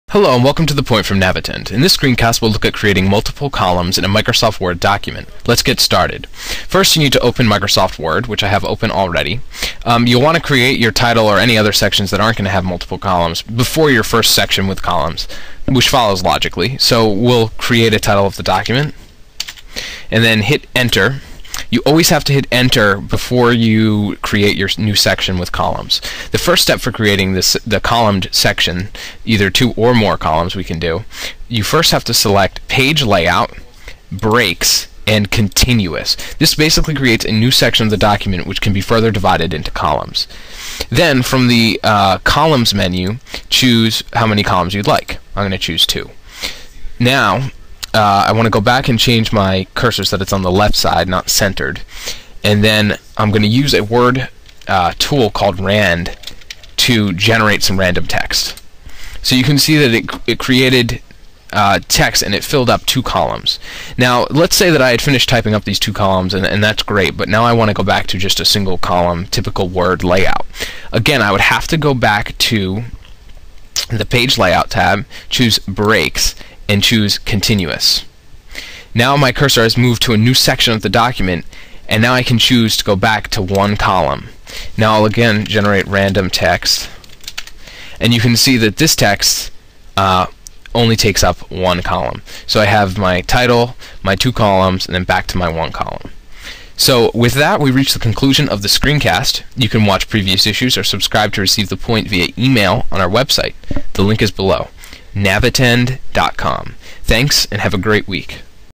How to Setup Multiple Columns in Microsoft Word
How to create multiple columns in Microsoft Word. This screencast explains how to insert two or more columns into your Microsoft Word document layout.